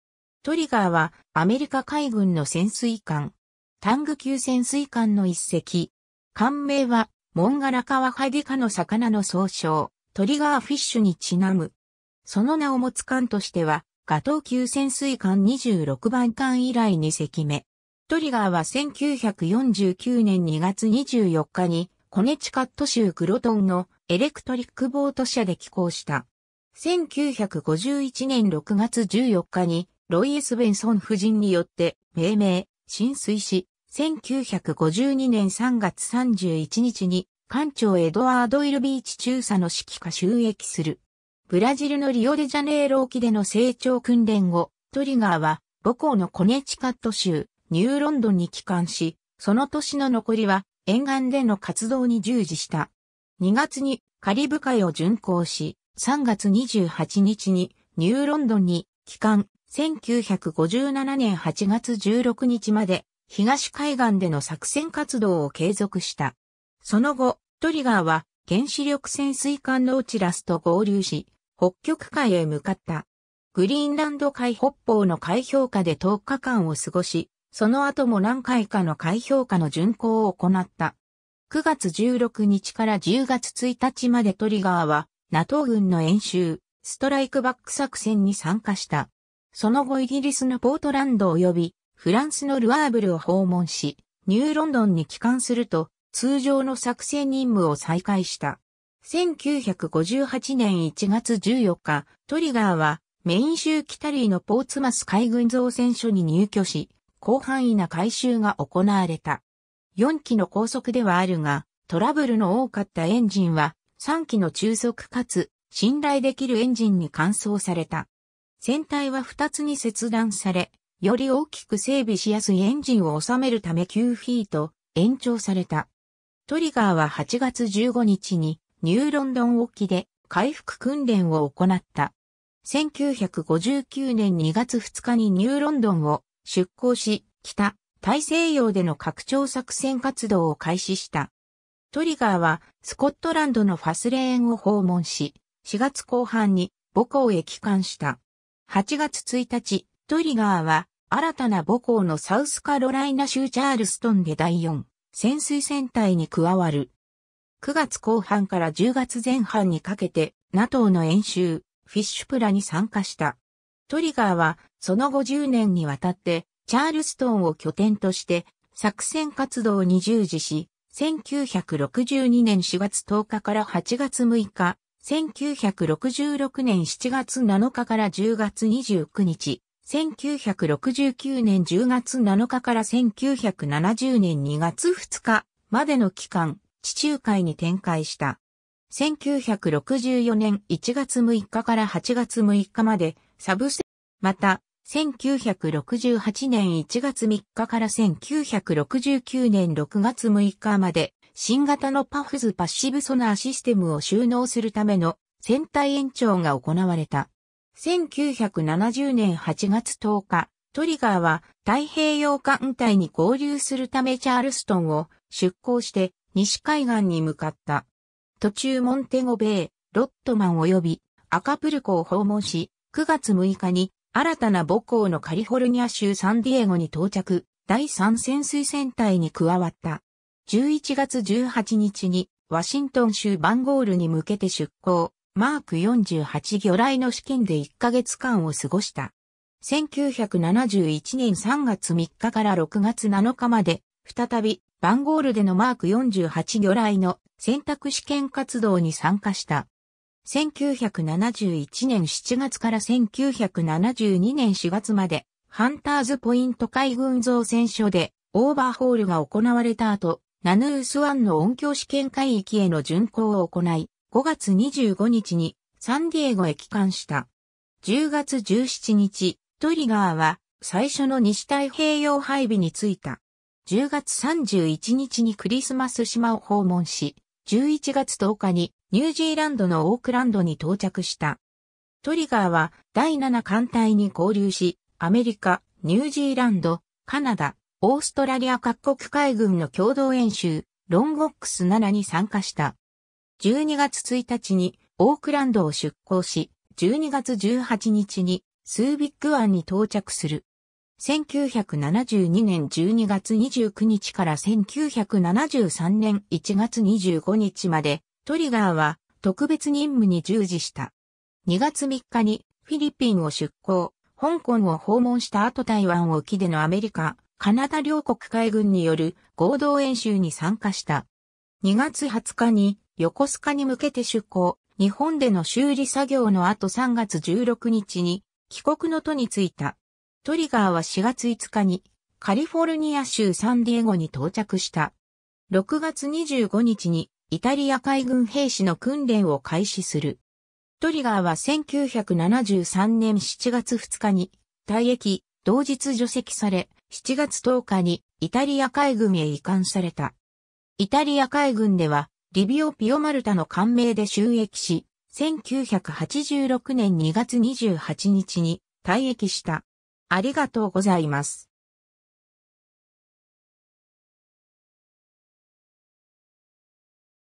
トリガー (SS-564)
トリガー (USS Trigger, SS-564) は、アメリカ海軍の潜水艦。
タング級潜水艦の1隻。
艦名はモンガラカワハギ科の魚の総称、トリガーフィッシュに因む。
その名を持つ艦としてはガトー級潜水艦26番艦(SS-237)以来2隻目。
 トリガーは1949年2月24日にコネチカット州グロトンのエレクトリック・ボート社で起工した。
1951年6月14日にロイ・S・ベンソン夫人によって命名、進水し、1952年3月31日に艦長エドワード・L・ビーチ中佐の指揮下就役する。
 ブラジルのリオデジャネイロ沖での整調訓練後、トリガーは母港のコネチカット州ニューロンドンに帰還し、その年の残りは沿岸での活動に従事した。
2月にカリブ海を巡航し、3月28日にニューロンドンに帰還、1957年8月16日まで東海岸での作戦活動を継続した。
その後、トリガーは原子力潜水艦ノーチラス (USS Nautilus, SSN-571) と合流し、北極海へ向かった。
グリーンランド海北方の海氷下で10日間を過ごし、その後も何回かの海氷下の巡航を行った。
9月16日から10月1日までトリガーはNATO軍の演習「ストライクバック作戦」に参加した。
その後イギリスのポートランドおよびフランスのルアーブルを訪問し、ニューロンドンに帰還すると通常の作戦任務を再開した。
 1958年1月14日、トリガーはメイン州キタリーのポーツマス海軍造船所に入渠し、広範囲な改修が行われた。
4基の高速ではあるがトラブルの多かったエンジンは、3基の中速かつ信頼できるエンジンに換装された。
船体は二つに切断され、より大きく整備しやすいエンジンを収めるため9フィート延長された。
トリガーは8月15日にニューロンドン沖で回復訓練を行った。
1959年2月2日にニューロンドンを出航し、北大西洋での拡張作戦活動を開始した。
トリガーはスコットランドのファスレーンを訪問し、4月後半に母港へ帰還した。
 8月1日、トリガーは新たな母港のサウスカロライナ州チャールストンで第4潜水戦隊に加わる。
9月後半から10月前半にかけてNATOの演習「Fishplay」に参加した。
トリガーはその後10年にわたってチャールストンを拠点として作戦活動に従事し、1962年4月10日から8月6日、1966年7月7日から10月29日、1969年10月7日から1970年2月2日までの期間、地中海に展開した。
1964年1月6日から8月6日まで、SUBSAFE プログラムに従った改修が行われた。
また、1968年1月3日から1969年6月6日まで、新型の PUFFS パッシブソナー・システムを収納するための船体延長が行われた。
 1970年8月10日、トリガーは太平洋艦隊に合流するためチャールストンを出航して西海岸に向かった。
途中モンテゴ・ベイ、ロッドマンおよびアカプルコを訪問し、9月6日に新たな母港のカリフォルニア州サンディエゴに到着、第3潜水戦隊に加わった。
11月18日にワシントン州バンゴールに向けて出航、Mk48 魚雷の試験で1ヶ月間を過ごした。
1971年3月3日から6月7日まで、再びバンゴールでのMk48魚雷の選択試験活動に参加した。
1971年7月から1972年4月までハンターズ・ポイント海軍造船所でオーバーホールが行われた後、ナヌース湾の音響試験海域への巡航を行い、5月25日にサンディエゴへ帰還した。
 10月17日、トリガーは最初の西太平洋配備に就いた。
10月31日にクリスマス島を訪問し、11月10日にニュージーランドのオークランドに到着した。
トリガーは第7艦隊に合流し、アメリカ、ニュージーランド、カナダ、オーストラリア各国海軍の共同演習「Longex 7」に参加した。
12月1日にオークランドを出航し、12月18日にスービック湾に到着する。
1972年12月29日から1973年1月25日まで、トリガーは特別任務に従事した。
2月3日にフィリピンを出航、香港を訪問した後台湾沖でのアメリカ、カナダ両国海軍による合同演習に参加した。
2月20日に横須賀に向けて出航、日本での修理作業の後3月16日に帰国の途に就いた。
 トリガーは4月5日にカリフォルニア州サンディエゴに到着した。
6月25日にイタリア海軍兵士の訓練を開始する。
トリガーは1973年7月2日に退役、同日除籍され、7月10日にイタリア海軍へ移管された。
 イタリア海軍ではリビオ・ピオマルタ ...